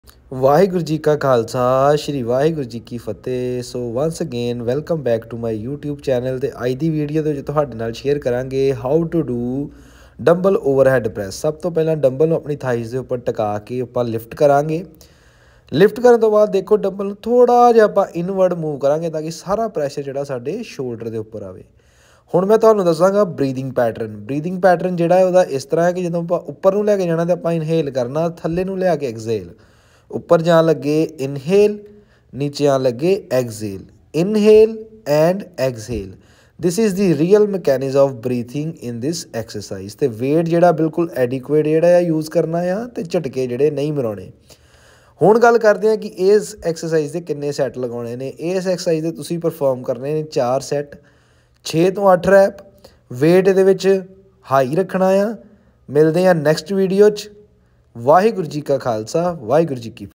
DB Overhead Press / right technique / sets and reps !! Info by fitnesswithharry
Waheguru g ka khalsa
Waheguru g ki  fateh

In this video we will learn about the performing mechanism of Db overhead press